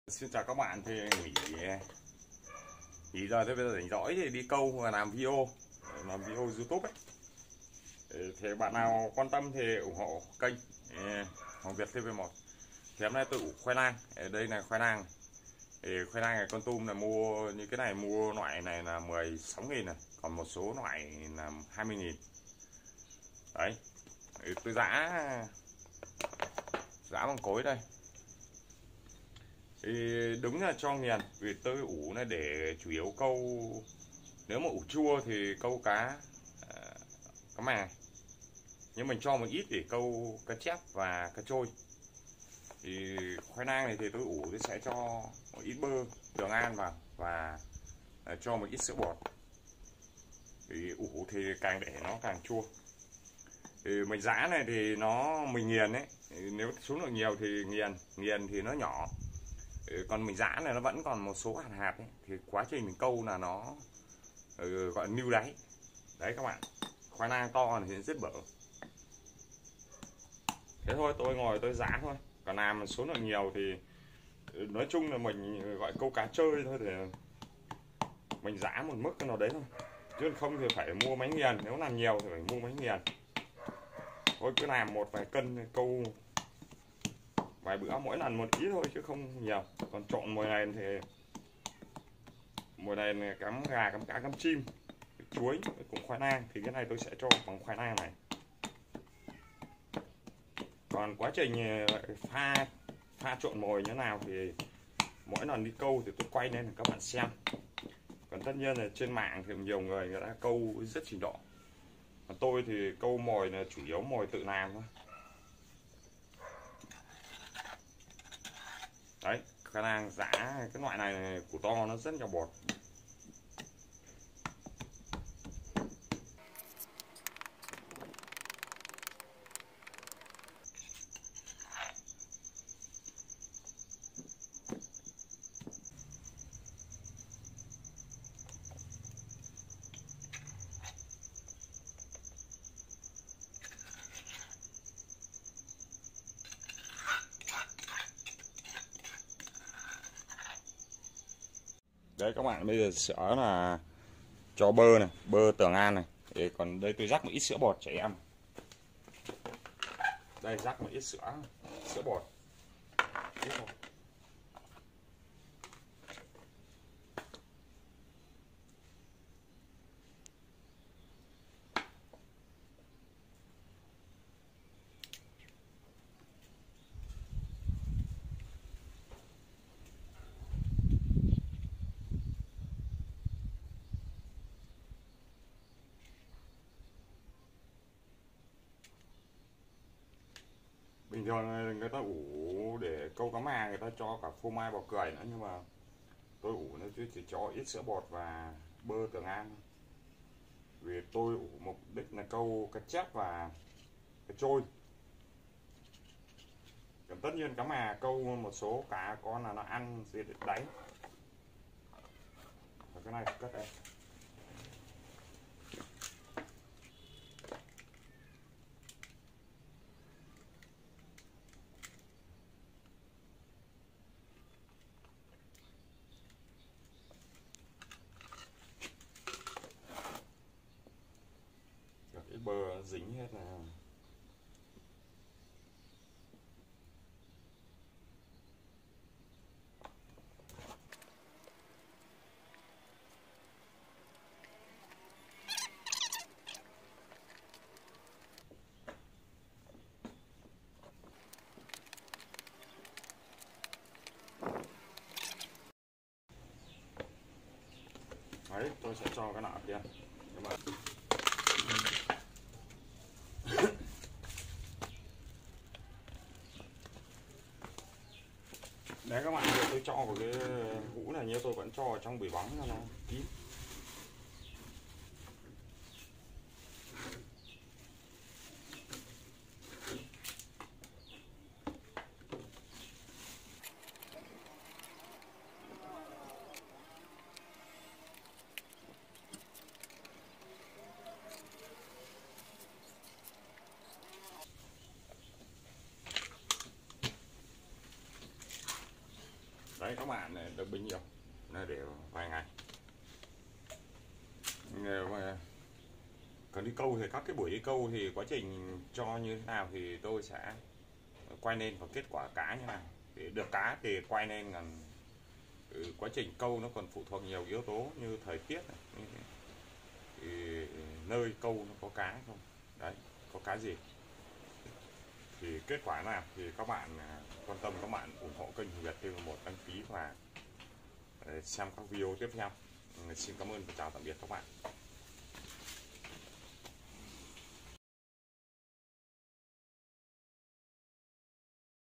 Cách Ủ Khoai Lang làm Mồi câu cá Chép, Trôi hồ tự nhiên
Mời các bạn cùng Du Lịch đến Kon Tum. Nhiều danh lam thắng cảnh đẹp thiên nhiên. Nhiều hồ tự nhiên, hồ thủy lợi, thủy điện, là những nơi câu cá giải trí tuyệt vời.
Video này tôi làm về Cách Ủ Khoai Lang làm Mồi câu cá chép, trôi hồ tự nhiên.
Kênh Hoàng Việt TV1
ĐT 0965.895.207